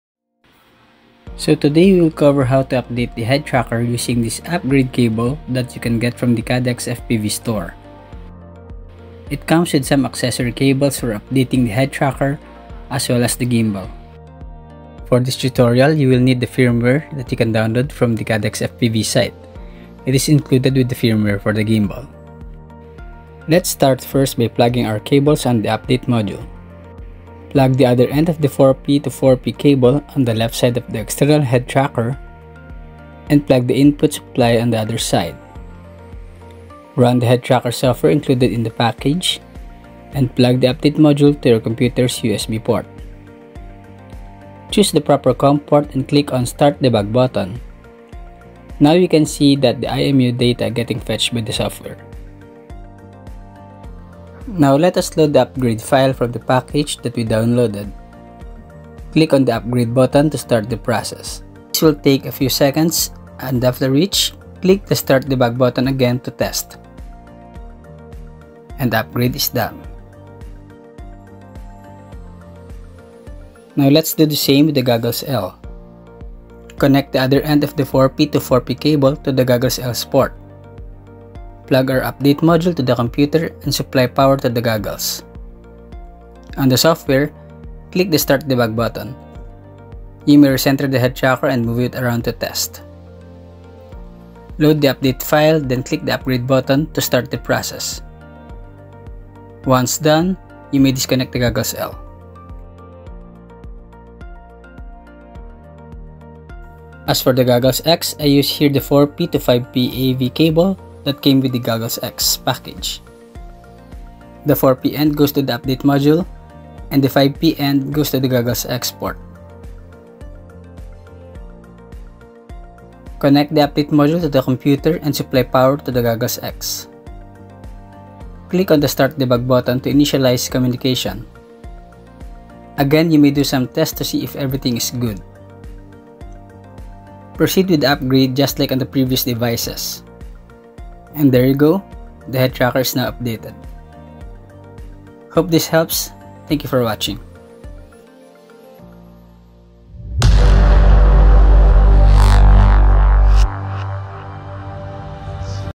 Walksnail Headtracker: How to update?
Here's a quick guide on how to update the firmware on the external head tracker (extension module) and the internal head tracker on either the Goggles L or new Goggles X.

Thanks again CaddxFPV for the unwavering support!